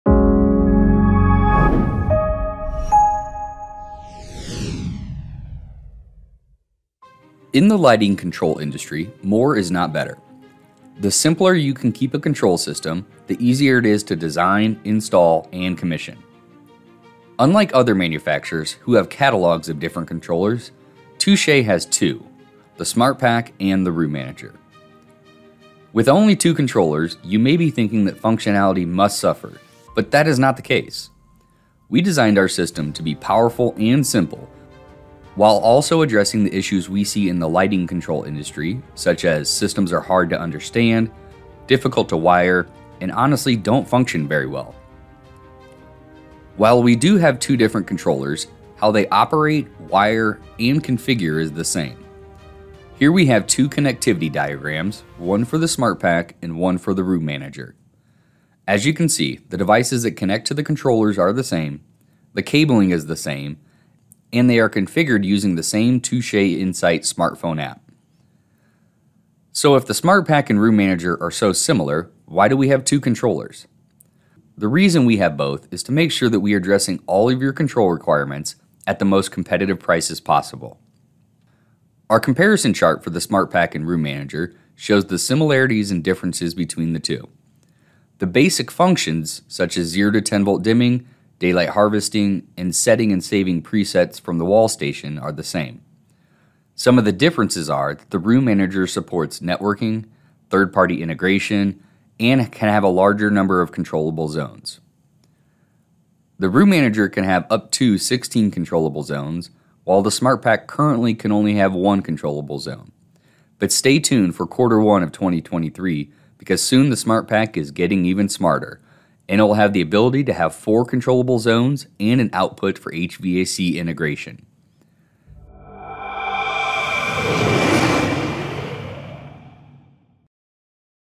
SmartPak vs. Room Manager: Simplified, Powerful Lighting Control Solutions by Touché
Discover how Touché redefines lighting control with its two intuitive controllers, the SmartPak and the Room Manager. Unlike other systems, Touché's streamlined approach eliminates the complexity of lighting control by focusing on simplicity and ease of use. Both controllers are designed to be powerful, easy to install, and efficient to configure, tackling industry issues like confusing wiring and poor functionality.

In this video, we highlight key features, similarities, and differences between the SmartPak and Room Manager. While both support essential functions like 0-10V dimming, daylight harvesting, and preset savings, the Room Manager stands out with advanced capabilities such as third-party integration, networking, and expanded control zones (up to 16 zones). The SmartPak, initially designed with one control zone, will soon support four zones and HVAC integration in early 2023.

With the Touché Insight app, both systems offer straightforward setups, making them adaptable and scalable for any control requirement at a competitive price.

Learn more about how Touché's streamlined solutions bring reliability and simplicity to lighting control.